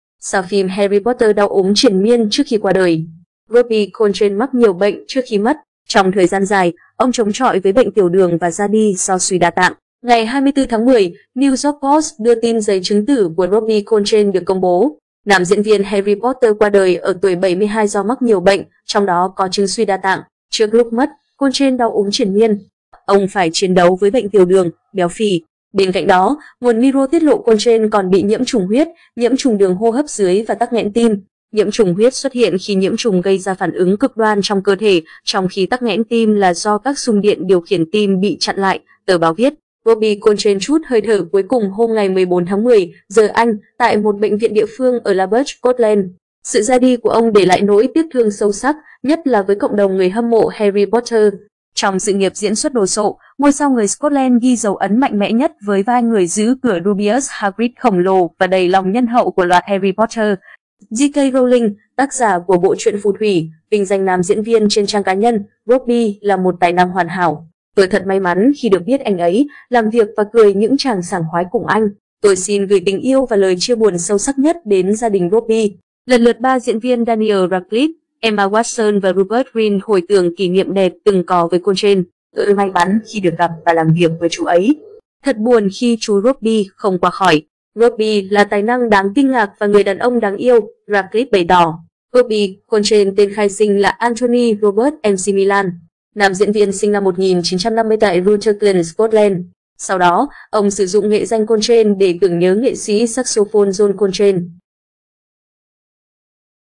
Sao phim 'Harry Potter' đau ốm triền miên trước khi qua đời
Robbie Coltrane mắc nhiều bệnh trước khi mất. Trong thời gian dài, ông chống chọi với bệnh tiểu đường và ra đi do suy đa tạng.Robbie Coltrane tên khai sinh là Anthony Robert McMillan. Nam diễn viên sinh năm 1950 tại Rutherglen, Scotland. Sau đó, ông sử dụng nghệ danh Coltrane để tưởng nhớ nghệ sĩ saxophone John Coltrane.
Robbie Coltrane trút hơi thở cuối cùng hôm 14/10 (giờ Anh) tại một bệnh viện địa phương ở Larbert, Scotland. Sự ra đi của ông để lại nỗi tiếc thương sâu sắc, nhất là với cộng đồng người hâm mộ Harry Potter.
 Robbie Coltrane (ngoài cùng bên trái) chứng kiến sự trưởng thành của các diễn viên nhí trong phim. Ảnh: NME.
Trong sự nghiệp diễn xuất đồ sộ, ngôi sao người Scotland ghi dấu ấn mạnh mẽ nhất với vai người giữ cửa Rubeus Hagrid khổng lồ và đầy lòng nhân hậu của loạt Harry Potter.
"Nhiễm trùng huyết xuất hiện khi nhiễm trùng gây ra phản ứng cực đoan trong cơ thể, trong khi tắc nghẽn tim là do các xung điện điều khiển tim bị chặn lại", tờ báo viết.
Ngày 24/10, New York Post đưa tin giấy chứng tử của Robbie Coltrane được công bố. Nam diễn viên Harry Potter qua đời ở tuổi 72 do mắc nhiều bệnh, trong đó có chứng suy đa tạng.
 Robbie Coltrane và Daniel Radcliffe trong Harry Potter.
Trước lúc mất, Coltrane đau ốm triền miên. Ông phải chiến đấu với bệnh tiểu đường, béo phì. Bên cạnh đó, nguồn Mirror tiết lộ Coltrane còn bị nhiễm trùng huyết, nhiễm trùng đường hô hấp dưới và tắc nghẽn tim.
Lần lượt 3 diễn viên Daniel Radcliffe, Emma Watson và Rupert Grint hồi tưởng kỷ niệm đẹp từng có với Coltrane. "Tôi may mắn khi được gặp và làm việc với chú ấy. Thật buồn khi chú Robbie không qua khỏi. Robbie là tài năng đáng kinh ngạc và người đàn ông đáng yêu", Radcliffe bày tỏ.
JK Rowling, tác giả của bộ truyện phù thủy, vinh danh nam diễn viên trên trang cá nhân: "Robbie là một tài năng hoàn hảo. Tôi thật may mắn khi được biết anh ấy, làm việc và cười những tràng sảng khoái cùng anh. Tôi xin gửi tình yêu và lời chia buồn sâu sắc nhất đến gia đình Robbie".